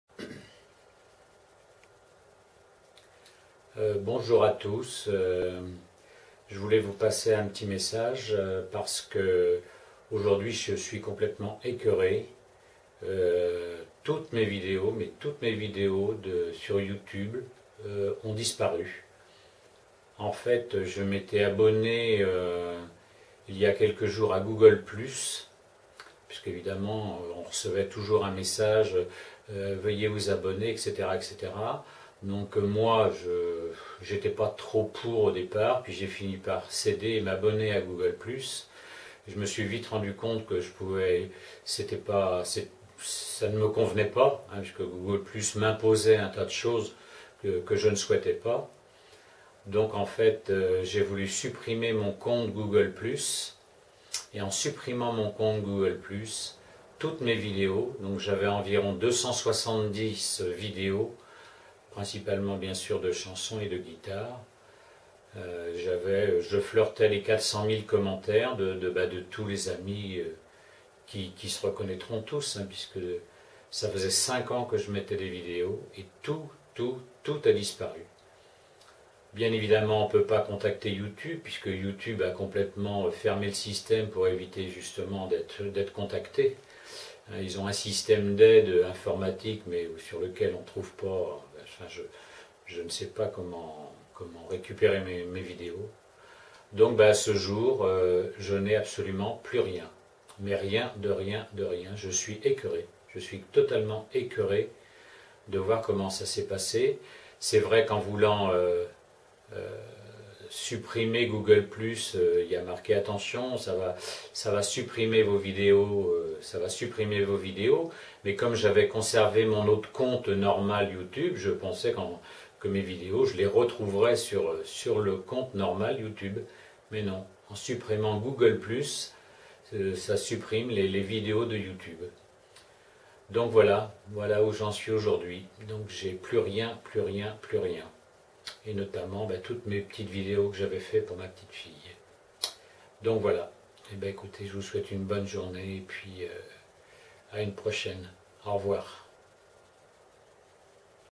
Vidéos supprimées
270 vidéos supprimées sur YouTube + tous les commentaires + tous les favoris et tout le reste. Aujourd'hui, je suis écoeuré car bien sur on ne peut pas contacter YouTube.
En voulant supprimer Google+, toutes mes vidéos soit 5 ans de travail et de liens créés avec vous tous sont partis en fumée;